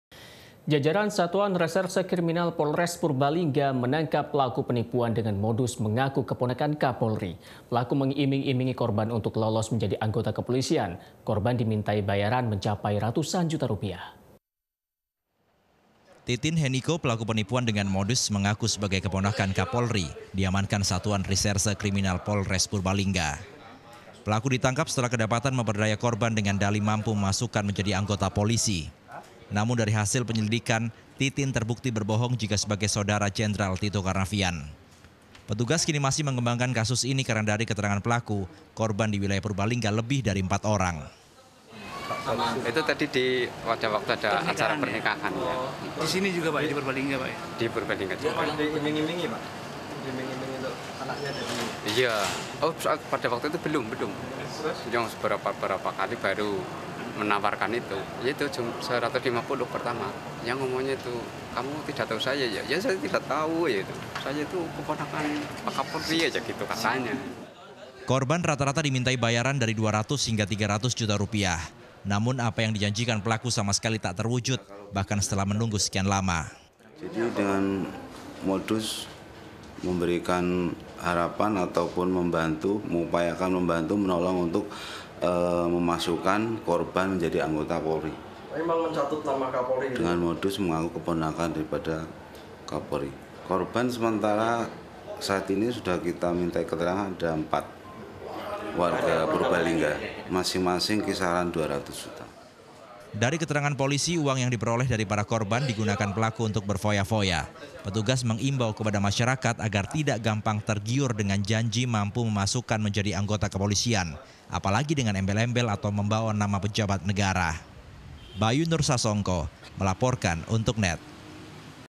Ngaku Jadi Keponakan Kapolri, Ini Loh Pelaku Penipuan Tes Masuk Polisi! - NET JATENG
Korban, rata-rata dimintai bayaran dari 200 hingga 300 juta rupiah. Namun, apa yang dijanjikan pelaku, sama sekali tak terwujud, bahkan setelah menunggu sekian lama.